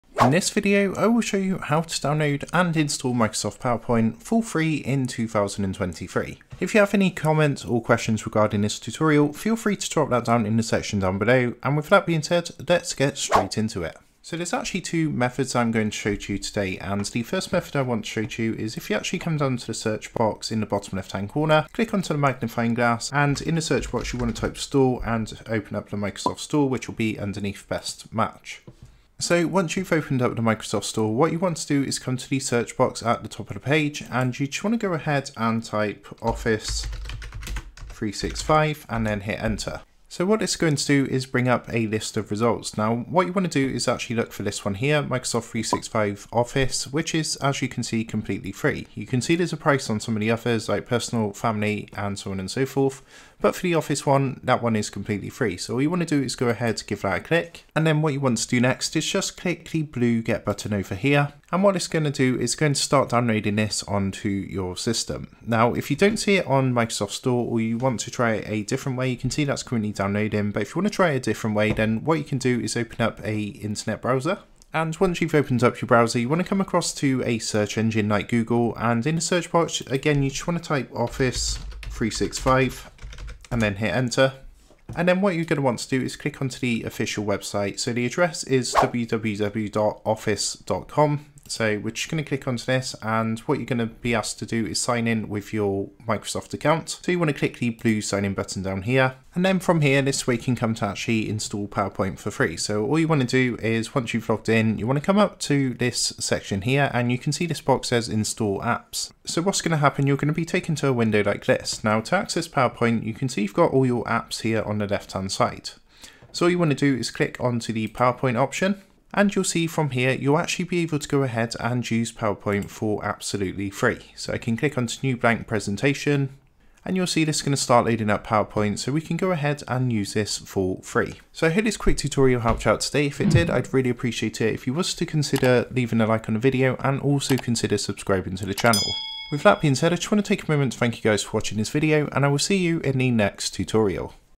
How To Download And Install Microsoft Powerpoint For Free 2023
In this video, we'll show you how to download and install Microsoft Powerpoint for free in 2023. Whether you're a student, a professional, or just someone who wants to create stunning presentations, Powerpoint is an essential tool that you need to have on your computer. We'll guide you through the entire process, from downloading the installation file to activating the software. You'll learn how to get the latest version of Powerpoint for free, without having to pay a single penny. So, if you're ready to take your presentation skills to the next level, watch this video and start using Powerpoint today!

 ✨Recommended VPN: ✨

Thanks!